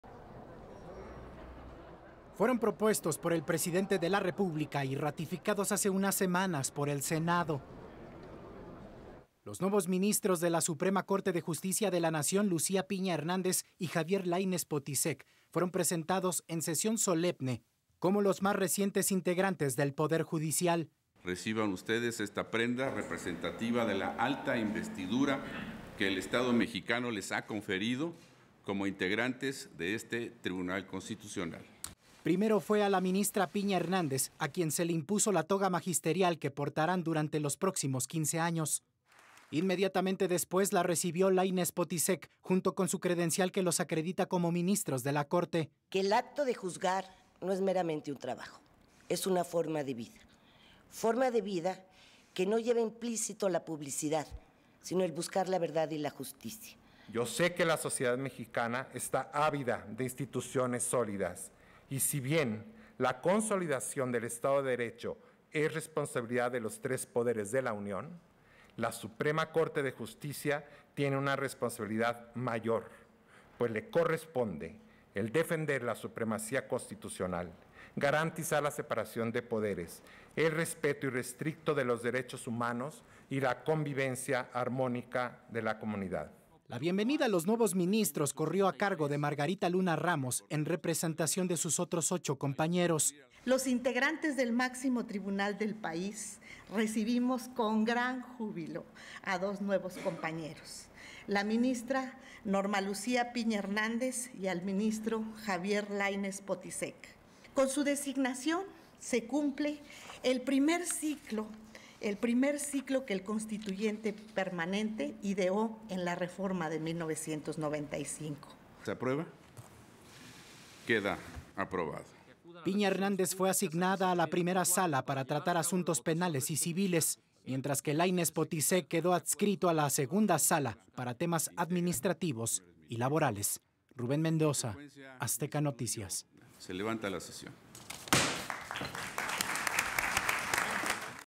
Corte da la bienvenida a 2 nuevos ministros | Noticias
Lucía Piña Hernández y Javier Laynez Potisek fueron presentados en sesión solemne como los más recientes integrantes del poder judicial.